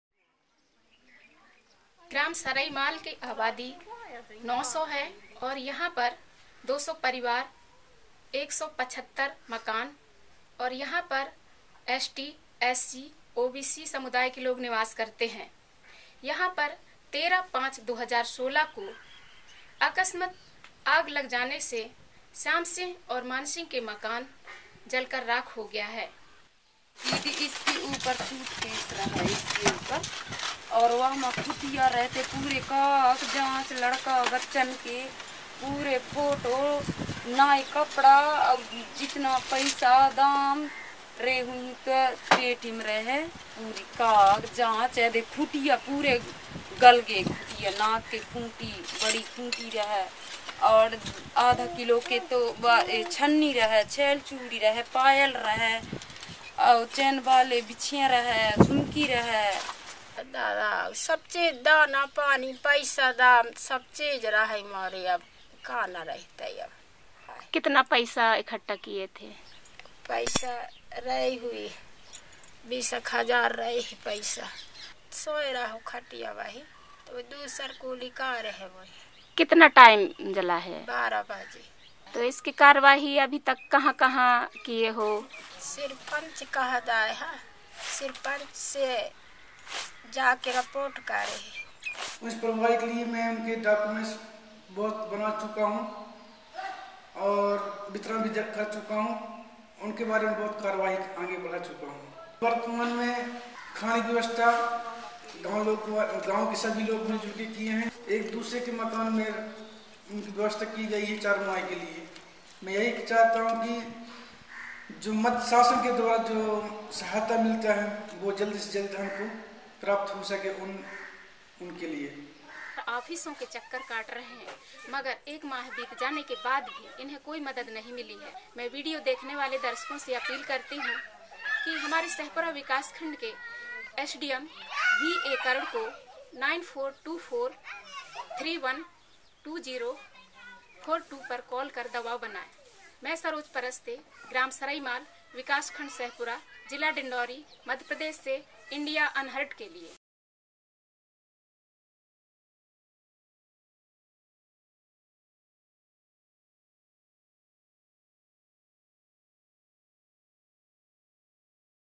Our home is destroyed in fire, Government has not yet supported us | Saroj reports IndiaUnheard
Community correspondent Saroj Paraste reports from Madhya Pradesh for Video Volunteers.